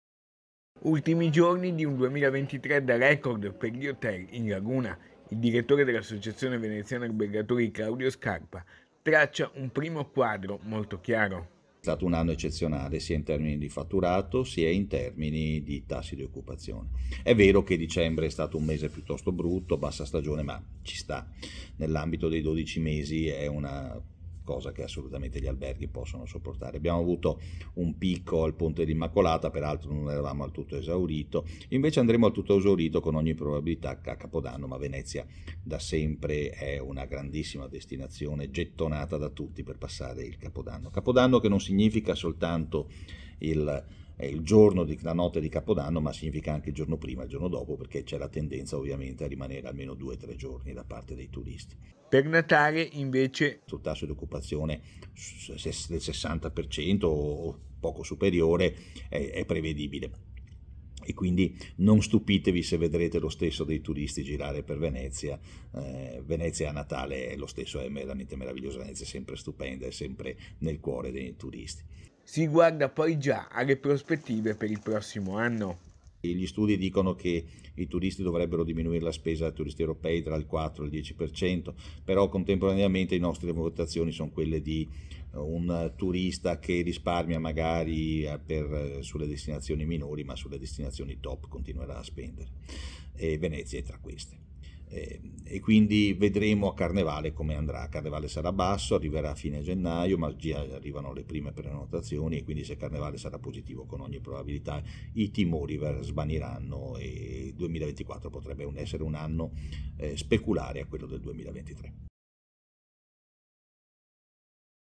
CAPODANNO DA TUTTO ESAURITO A VENEZIA | 22/12/2023
A3 NEWS Venezia 22/12/2023 - VENEZIA - Gli albergatori veneziani tracciano un primo bilancio del 2023 che per gli hotel in laguna è stato da record. A Natale occupazione delle camere attorno al 60 per cento, picco a Capodanno.  || Ultimi giorni per un 2023 da record per gli albergatori veneziani. Il direttore dell'associazione veneziana albergatori Claudio Scarpa traccia un quadro molto chiaro.Si guarda poi già alle prospettive per il prossimo anno.Gli albergatori veneziani tracciano un primo bilancio del 2023 che per gli hotel in laguna è stato da record. A Natale occupazione delle camere attorno al 60 per cento, picco a Capodanno. - Intervistati CLAUDIO SCARPA (DIR. ASSOCIAZIONE VENEZIANA ALBERGATORI) (Servizio di Lorenzo Mayer) - Segui Antenna Tre anche sul digitale terrestre! Visita il sito